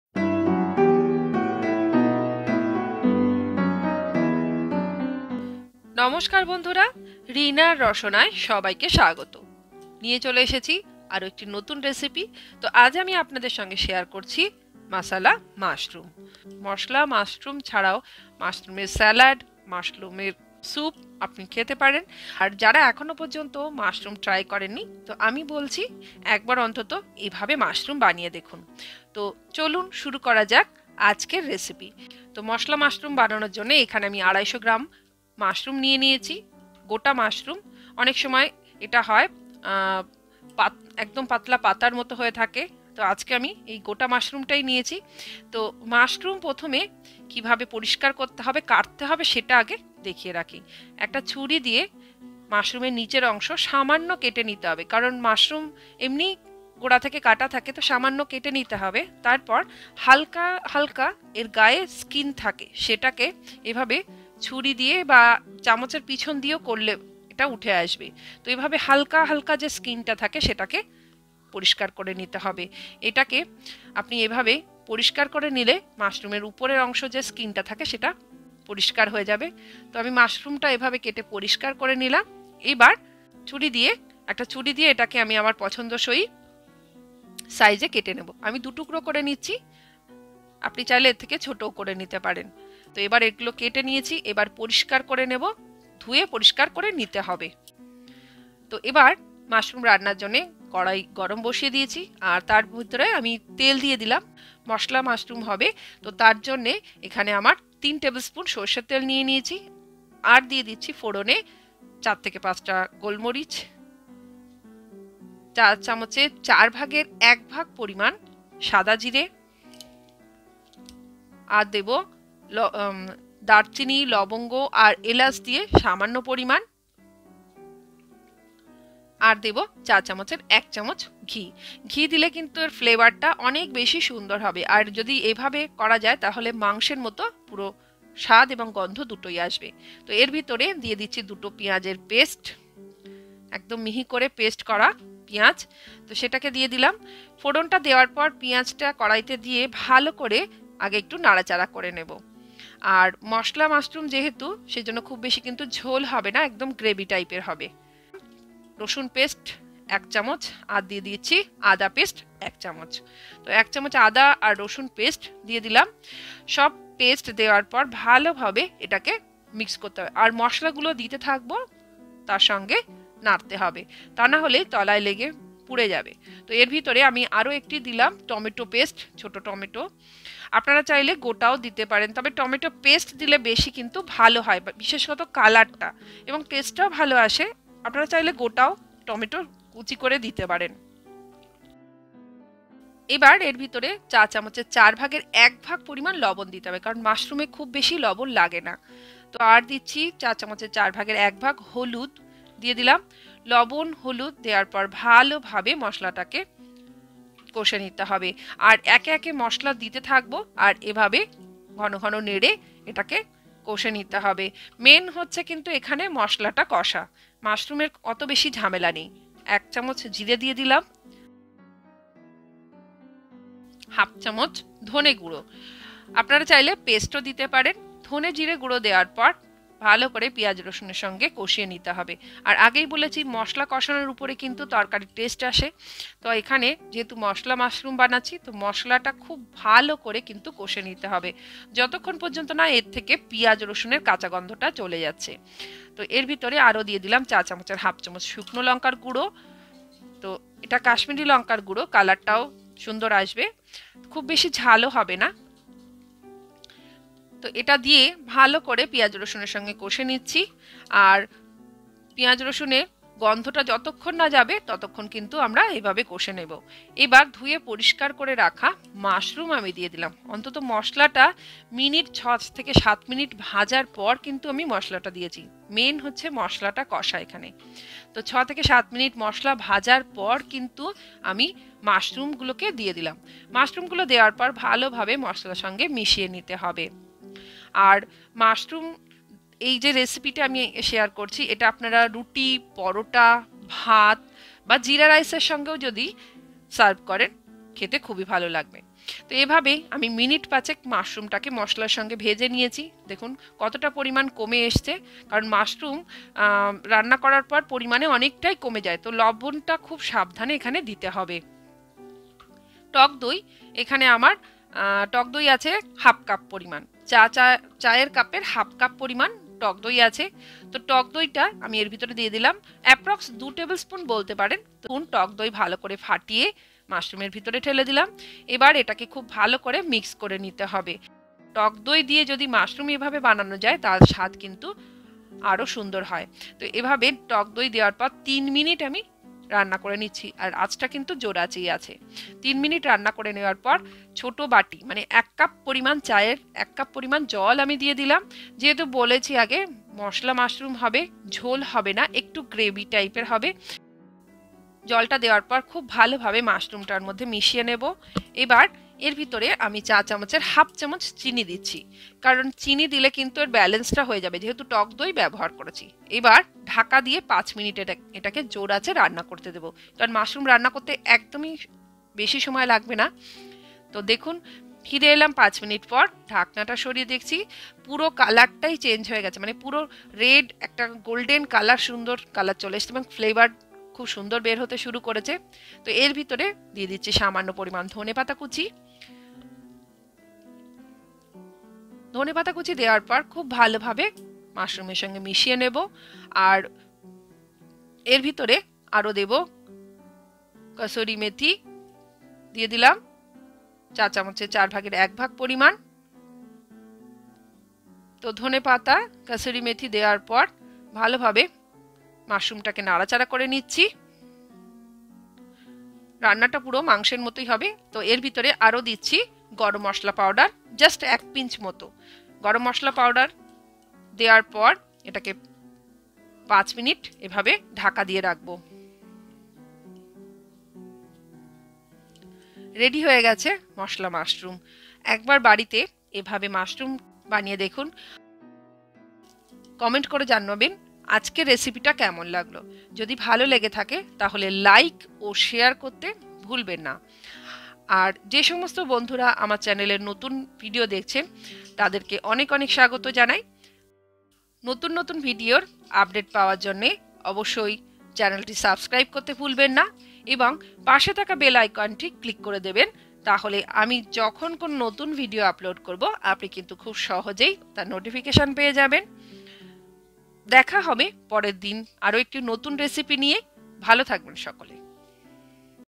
Restaurant-style mushroom masala--easy and quick mushroom recipe || Rinar Rasana
Today I will show you another delicious recipe, an easy and quick mushroom recipe. It is a very famous Bengali recipe. I will show you this famous recipe step by step.

Video Title: Restaurant-style mushroom masala--easy and quick mushroom recipe || Mushroom Masala || Rinar Rasana

I hope you will enjoy watching this video. I will try to answer my level best.